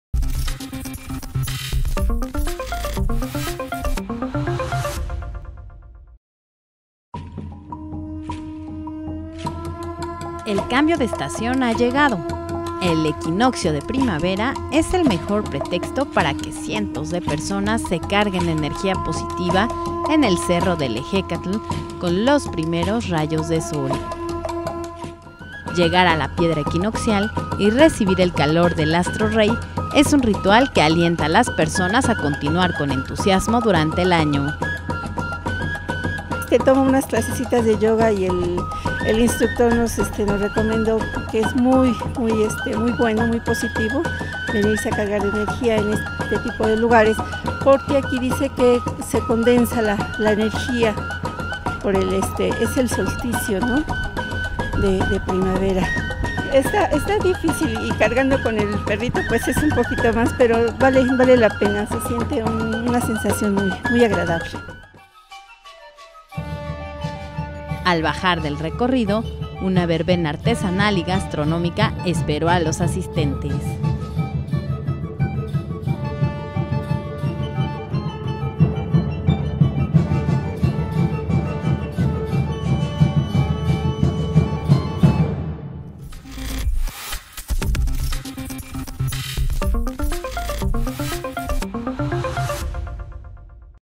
Reciben vecinos de Ecatepec primavera en el Ehécatl
Para cargarse de energía positiva, cientos de personas suben cada 20 y 21 de marzo a este emblemático cerro como parte del ritual de equinoccio de primavera